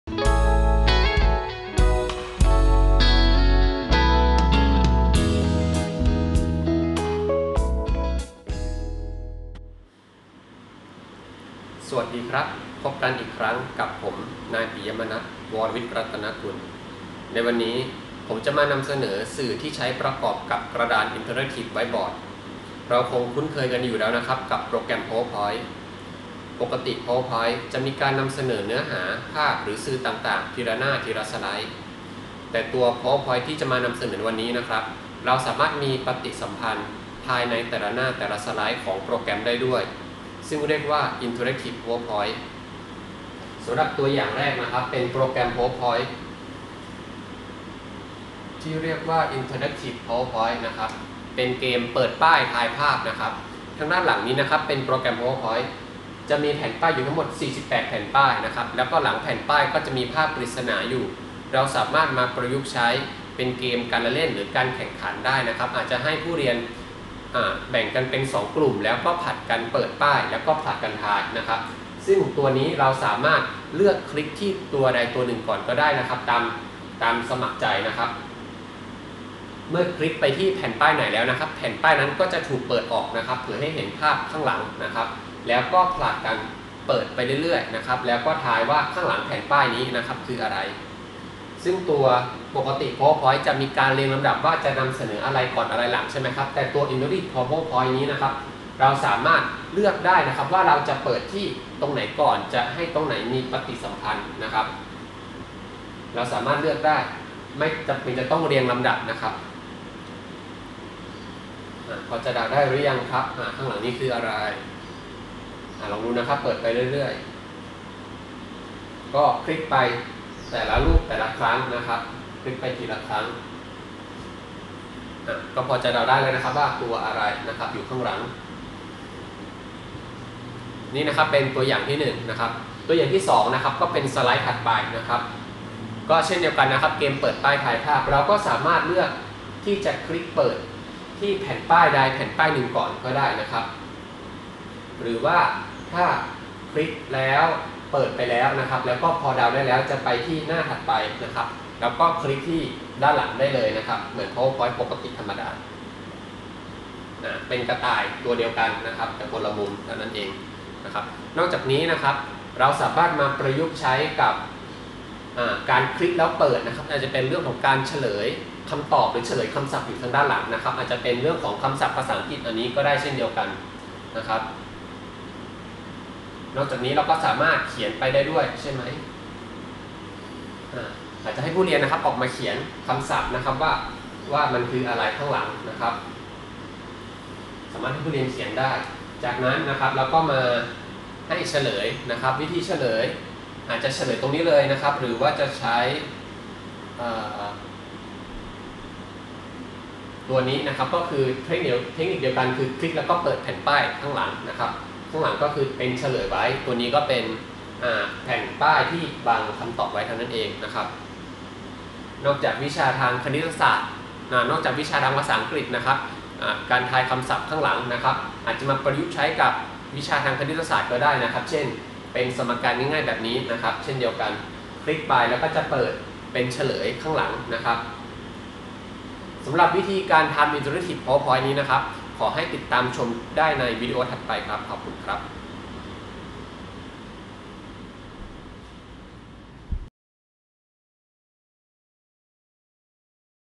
การใช้ Interactive PowerPoint ร่วมกับกระดาน IWB ตอนที่ 1
การใช้ Interactive PowerPoint ร่วมกับกระดาน IWB
- แนะนำตัวอย่าง Interactive PowerPoint